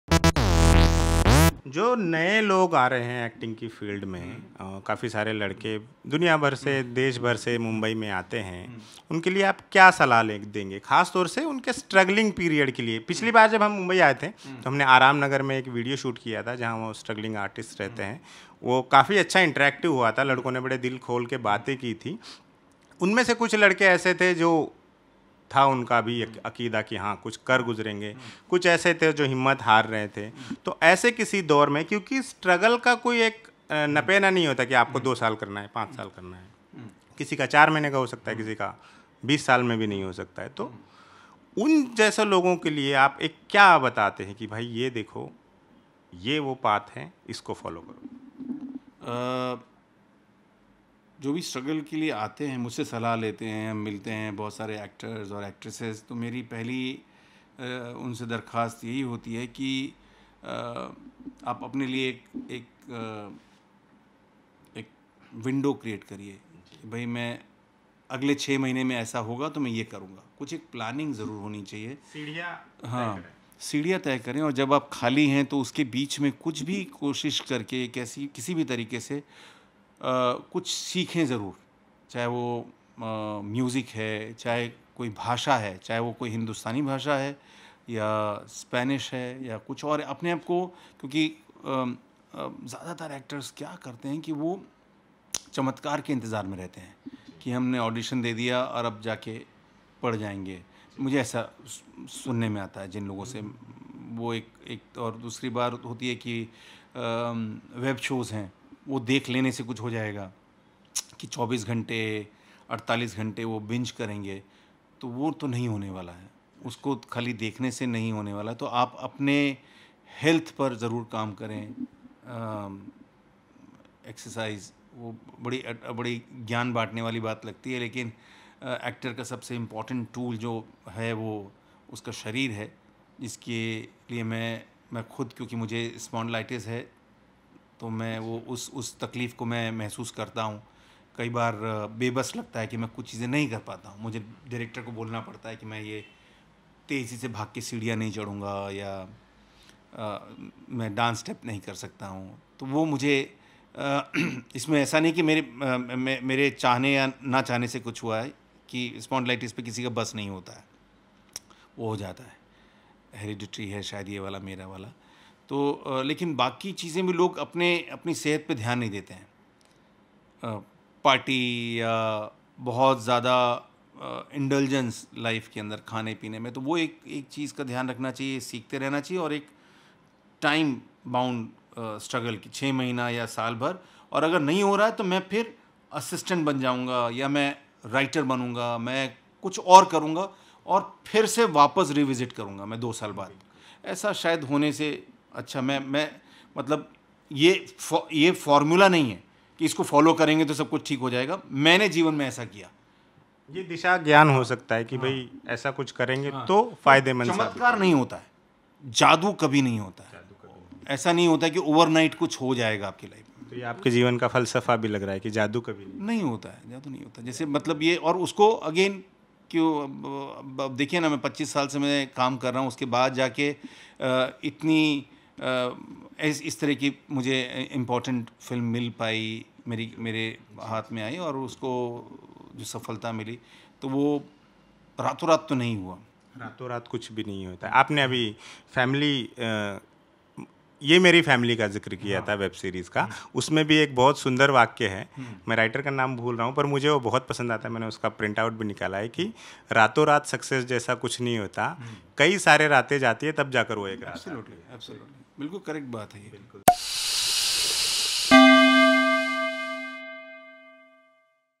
Acting में Struggle करने वालों को क्या करना चाहिए, Actor Gajraj Rao से सुनिए | Gajraj Rao Interview
During our Maharashtra visit, we have met actor Gajraj Rao in his Khar office. We had a lengthy chat about Gajraj Rao's journey, struggle, his work in different aspects of the cinema industry and his recent grand success. The full interview will be published soon. In this part, you can see Gajraj Rao's advice to struggling actors.

Edited By: Rashid ALi Kazmi